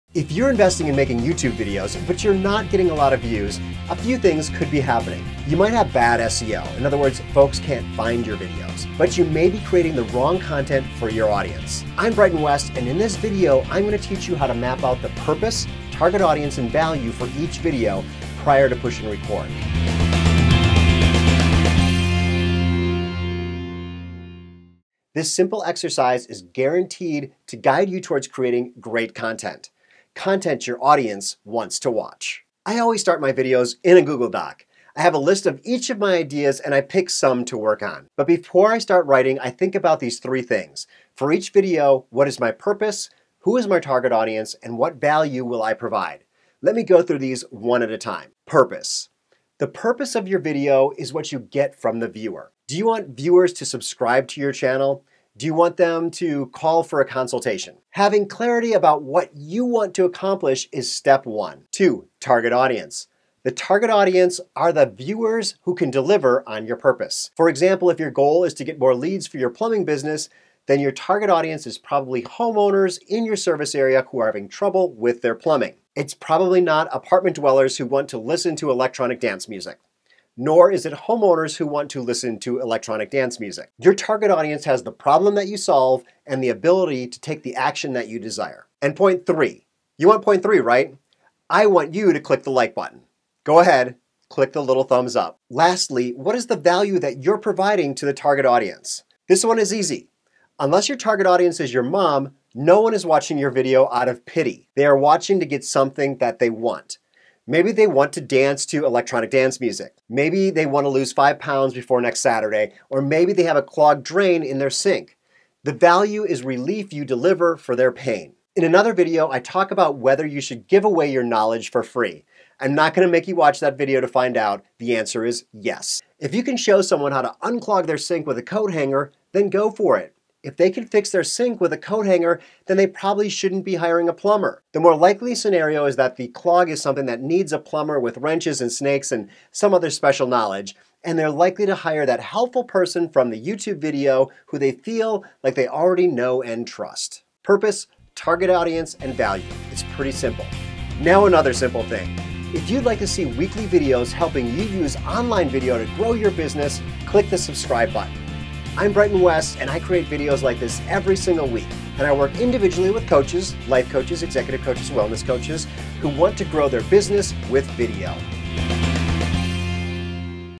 Create Smart Videos for Your Audience | Brighton West
Create great content and get more video views by mapping out these concepts before you push record. Have you been investing time and effort into making YouTube videos, but you don’t seem to be getting a lot of views?
It might be because of bad SEO, but you might also be creating the wrong content for your audience.

In this video, I’m going to show you how you can create great content by mapping out these three things: purpose, target audience and value.

When it comes to creating content, I don’t just sit in front of the camera and talk about whatever comes to mind. I have a Google Doc where I list my ideas and pick a few to work on.

This is where purpose, target audience and value come in.

PURPOSE
Do you want them to share your video with their friends?

This answers the question, “What do you get from the viewer?” Before you start anything, you should be clear about what you want to accomplish.

TARGET AUDIENCE
If you’re a plumber, your target audience are homeowners with plumbing problems. If you’re a business coach, then your target market could be new entrepreneurs who want to accelerate their business growth.

Your target audience are your viewers who can deliver on your purpose. They have the problem your video can solve.

VALUE
The value is what your target audience gets from watching the video. This is you helping them overcome their pain point.

Continuing with the plumber example, you can give value by creating videos that show different ways on how to unclog a sink.

Not only does it position you as an expert; it also shows that you know what you’re doing, and people are more likely to hire you because of it.

Are you ready to implement these three things into your content strategy?

Follow these 3 tips to get more video views on YouTube